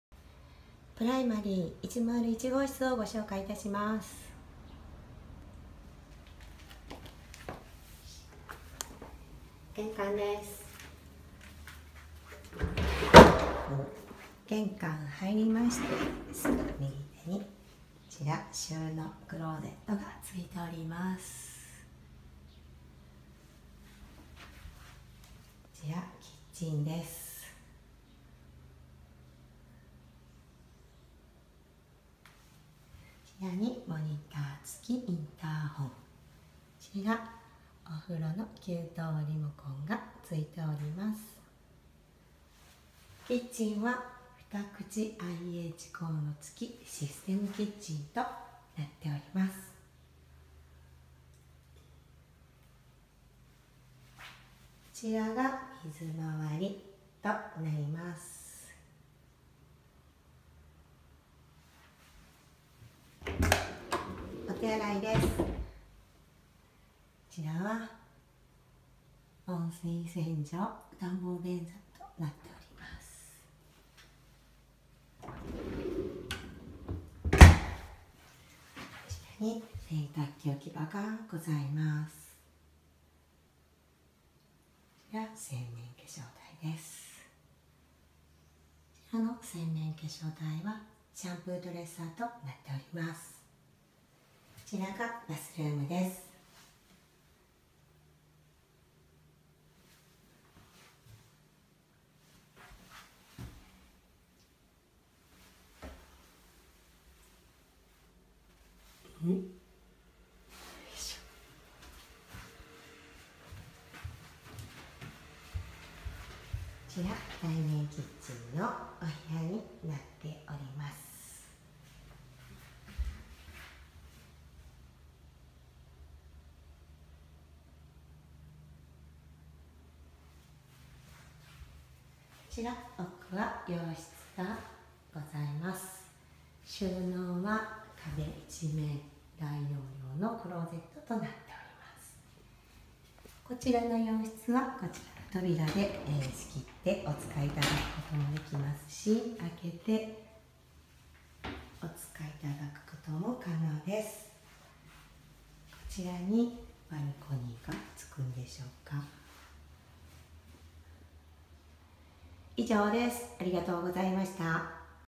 奈良県橿原市で賃貸をお探しの方は【ならすも】プライマリー101橿原神宮前駅1ＬＤＫ
平成29年9月末完成予定の新築ハイツです！！橿原神宮前まで10分ですので、通勤で電車を使う方にもおすすめです！！